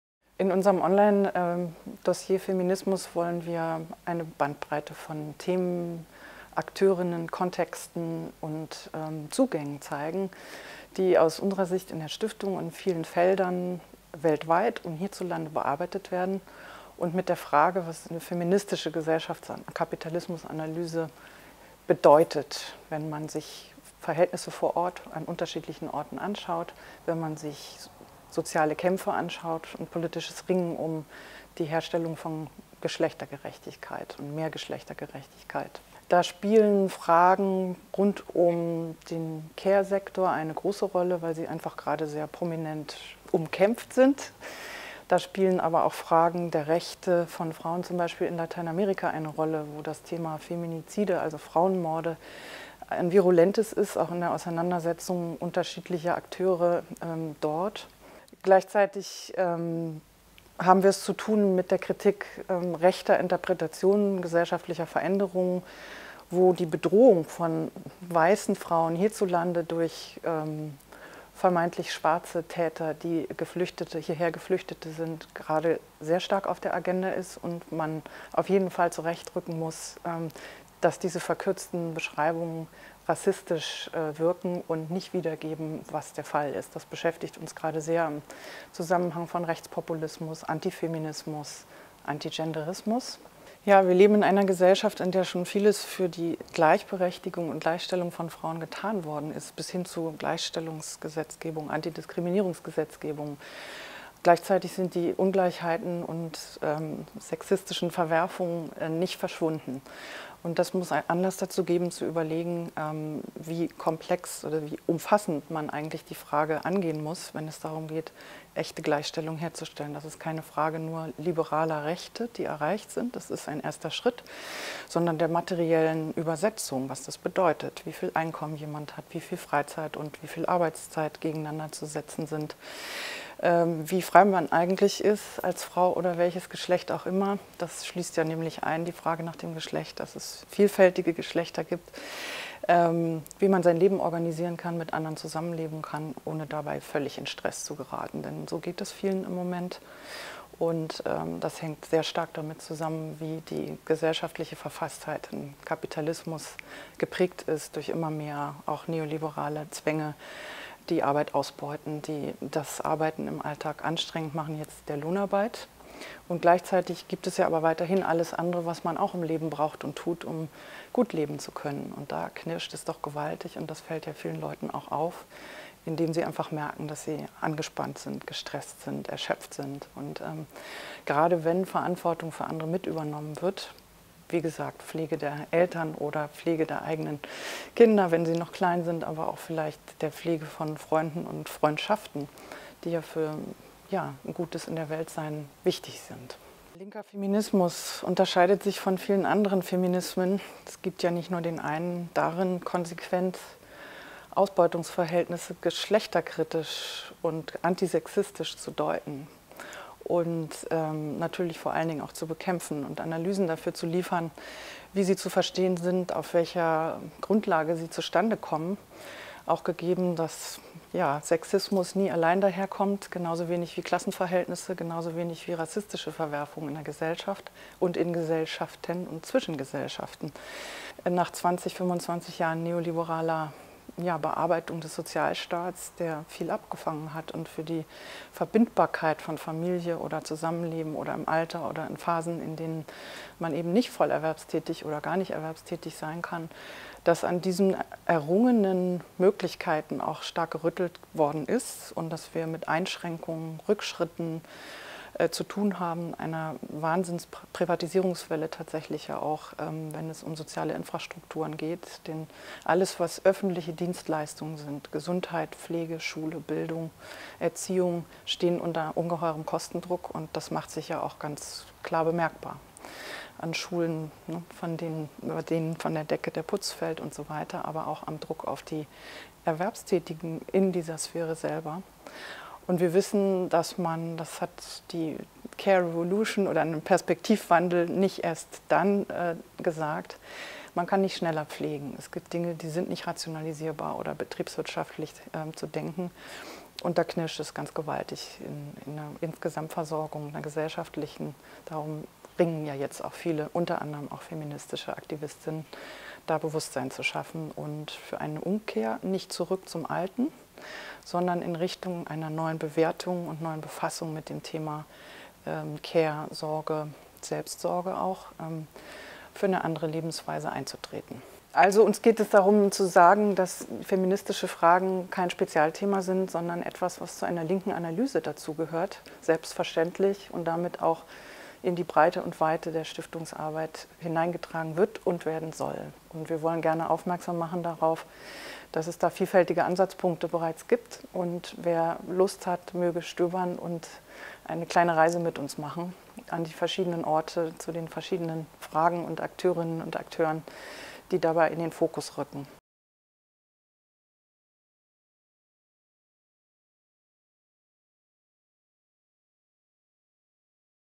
Queer-feministische Kämpfe für eine solidarische Gesellschaft
Katharina Pühl, Referentin für feministische Gesellschafts- und Kapitalismuskritik der Rosa-Luxemburg-Stiftung, stellt den Themenschwerpunkt «Feminismus von links» vor.